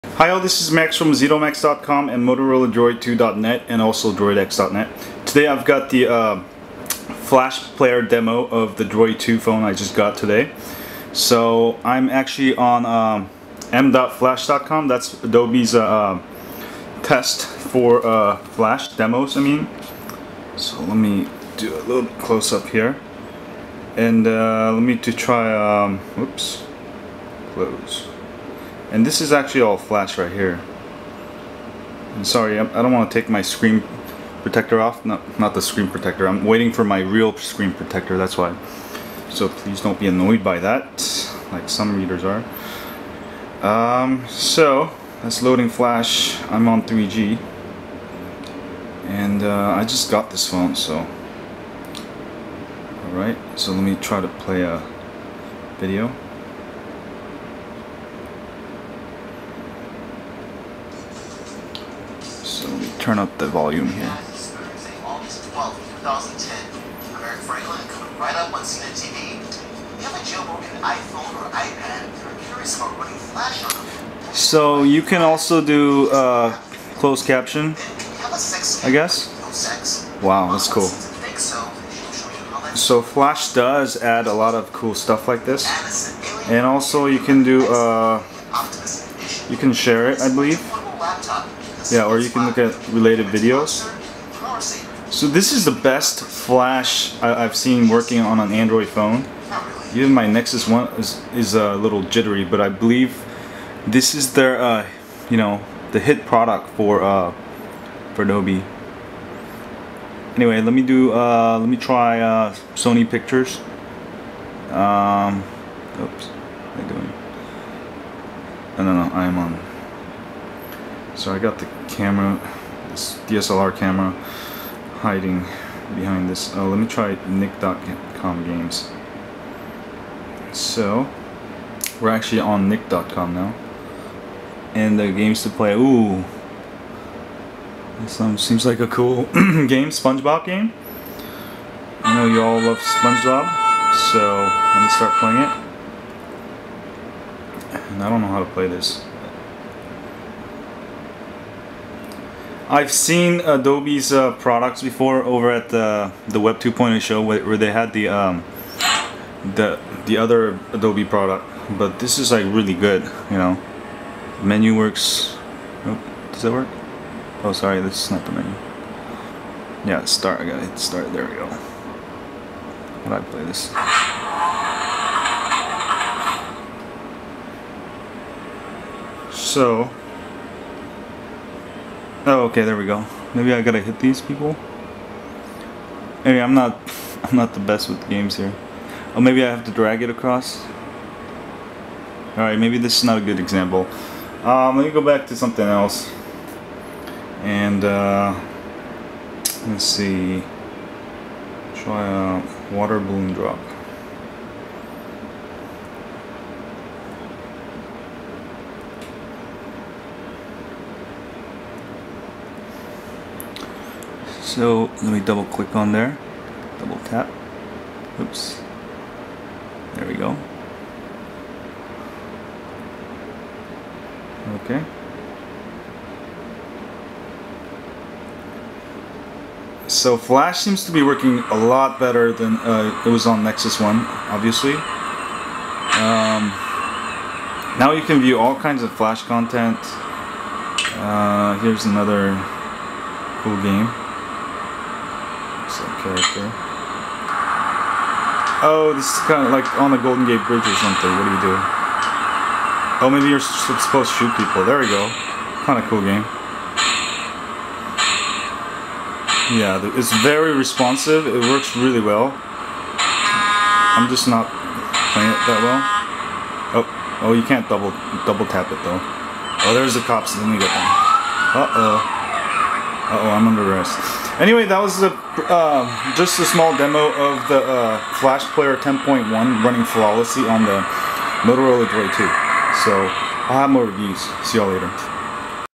Droid 2 Review - Flash Player 10.1 Demo!
Here's a quick review of the new Droid 2's Flash Player 10.1 running flawlessly.